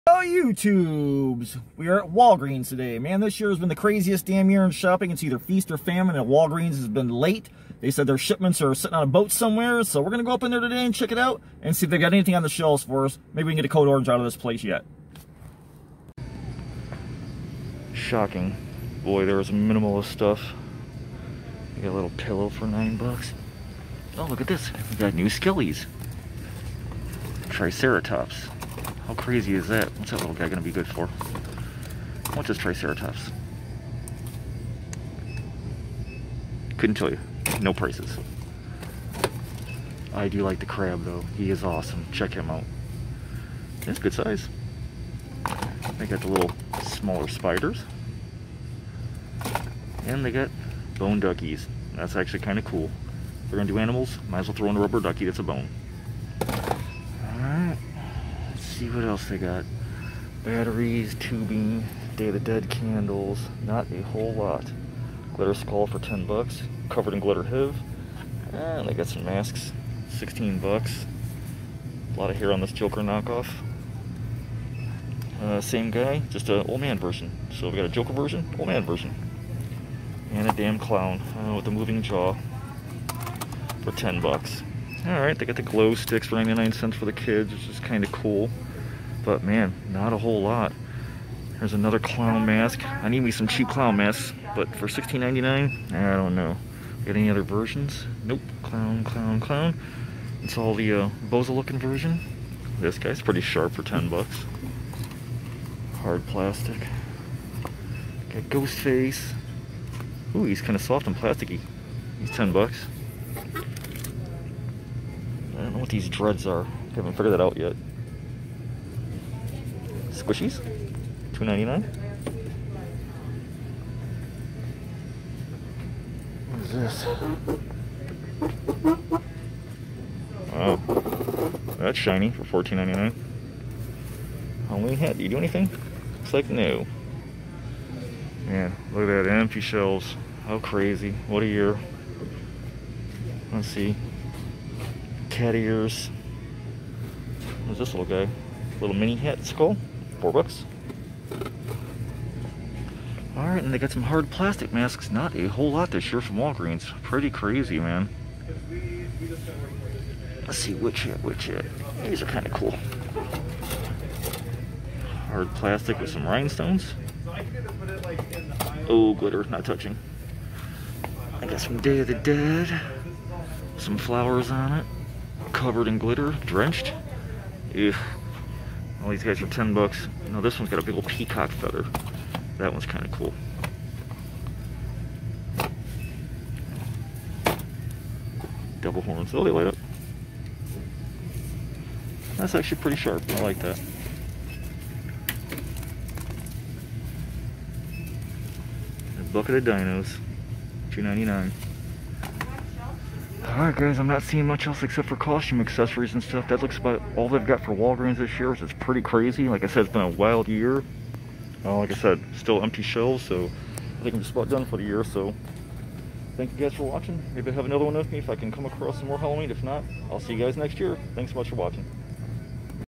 Walgreens Halloween 2021! Masks and new skellies!| Cobwebs And Candlesticks
OK Haunt Fam! I perpetrate up in Walgreens for the 7th time to finally be greeted with Halloween props and decor! They said shipments were a month late but we finally got dome black and orange on the shelves with new animal skellies! Have a look for yourself!

You should check out this Etsy shop